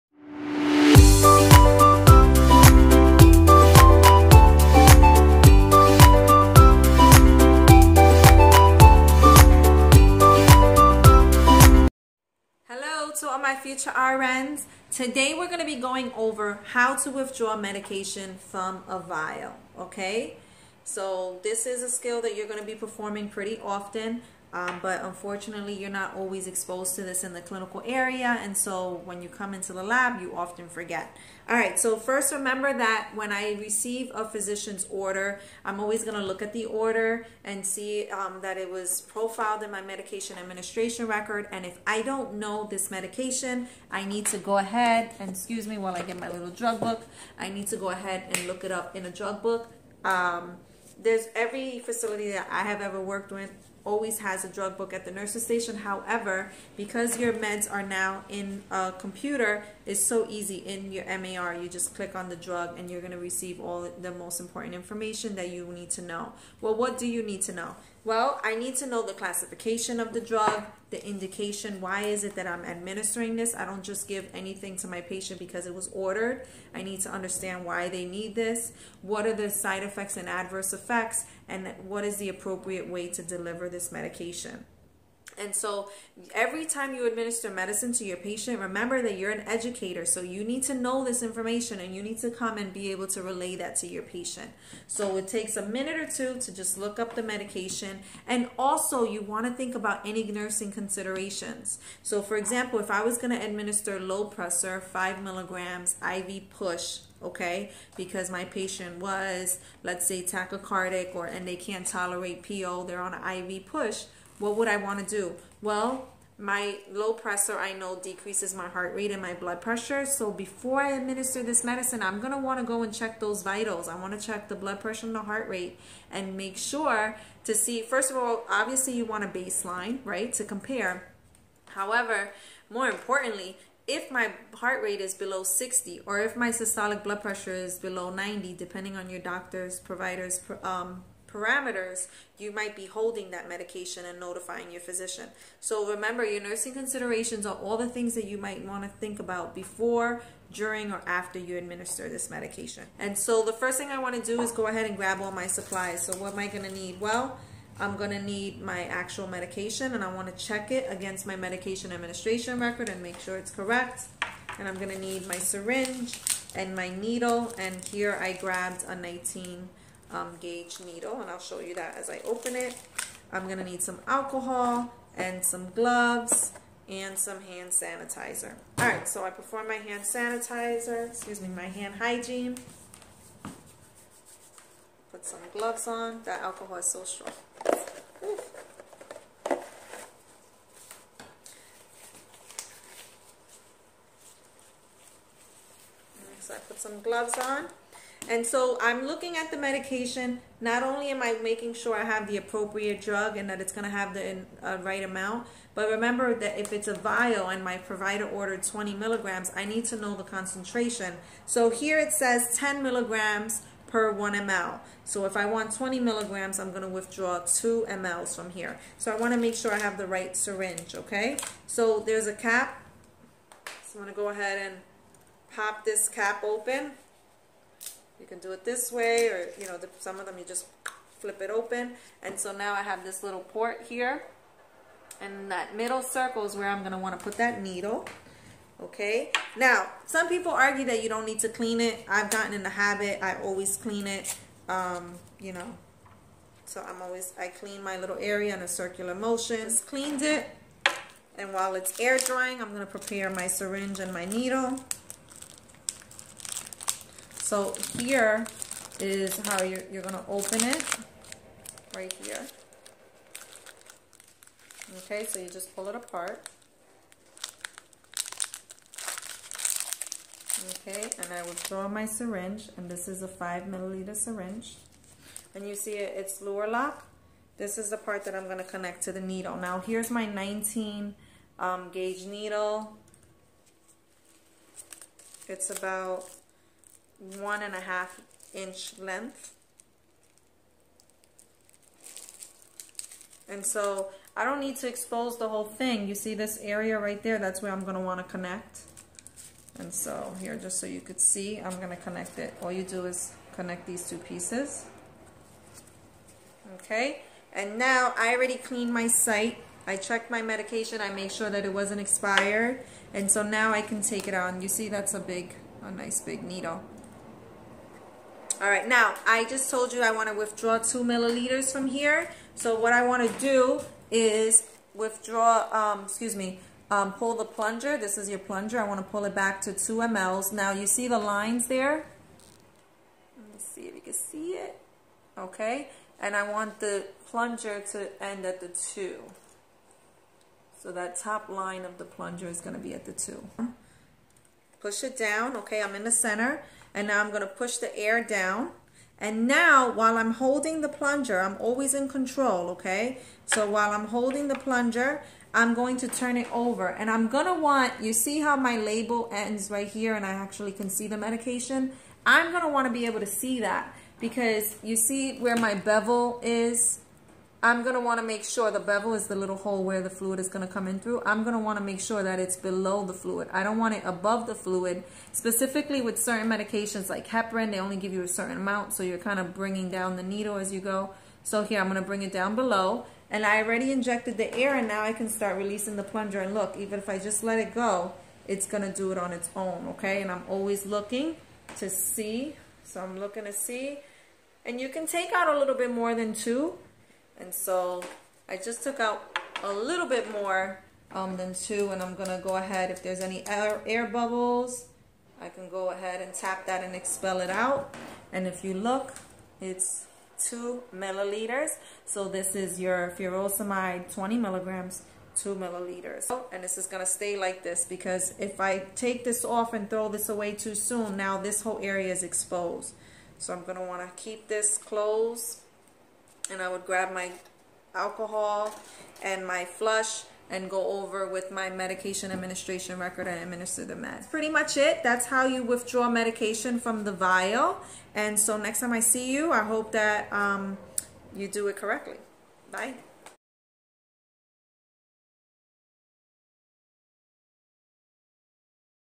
How to withdraw medication from a vial???
Step by step medication withdrawal from a vial.

Today’s reference for this skill:
Taylor's Clinical Nursing Skills

This video is intended for healthcare professionals. As always be sure to follow your facility policy and/or textbook as your reference.